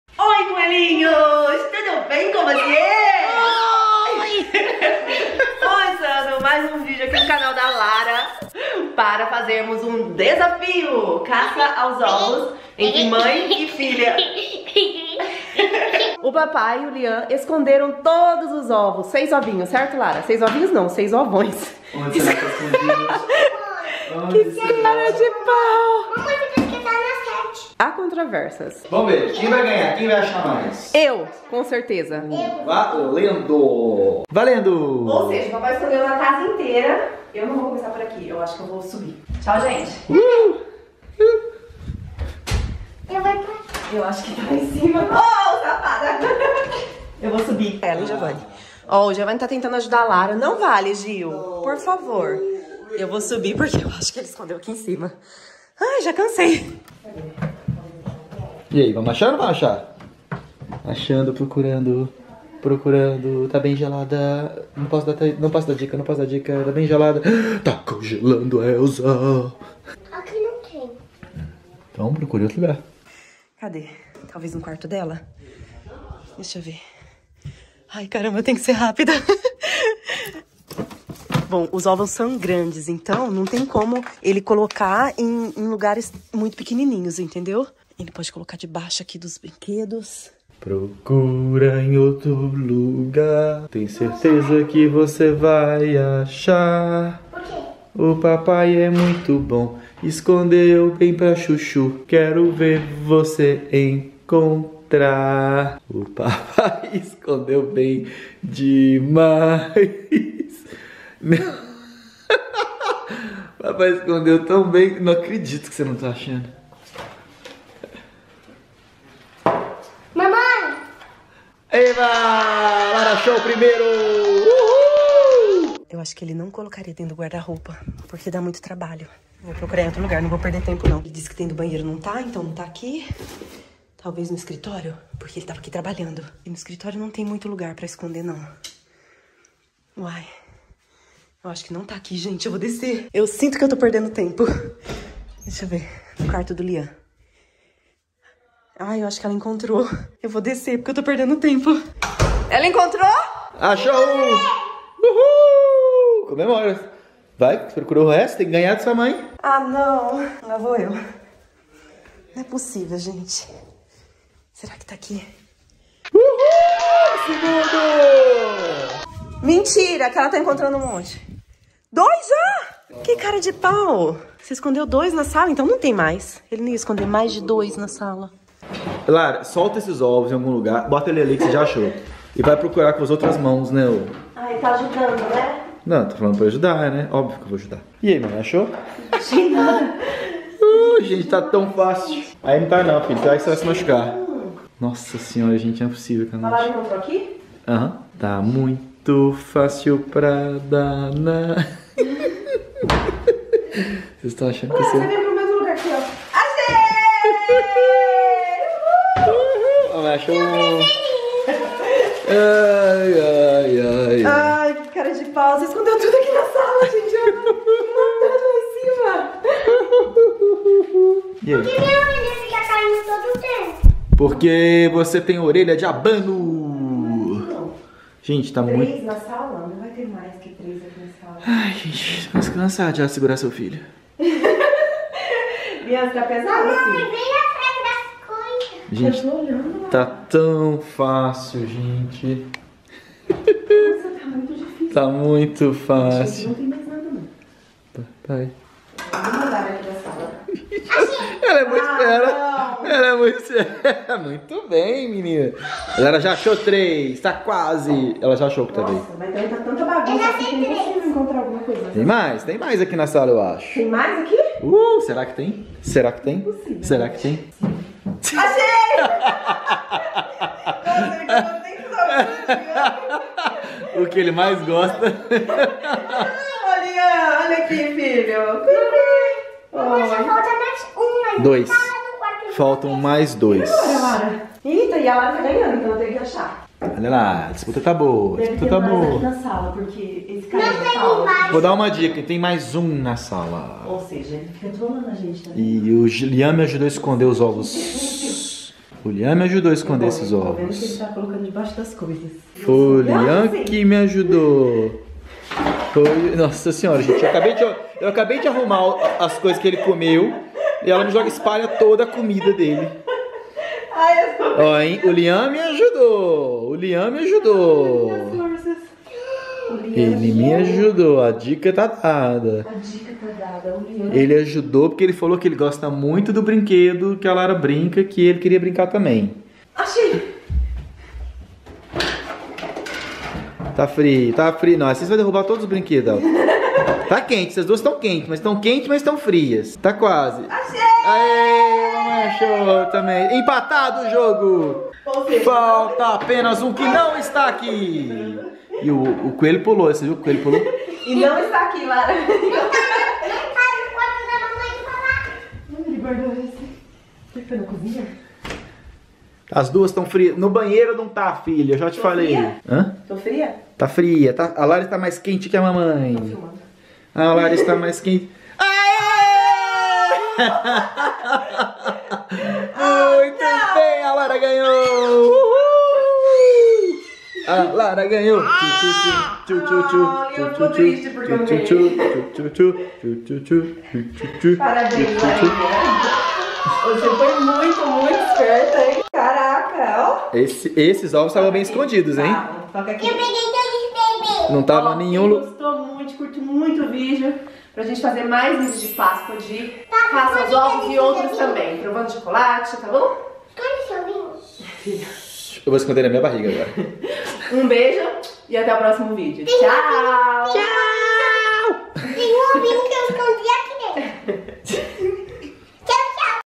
CAÇA AOS OVOS DE PÁSCOA - Lara e família
Larinha e mamãe na disputa pra ver quem encontra mais ovos de páscoa!

Se você ama série, música, jogos, desafios e diversão em familia, esta no canal certo!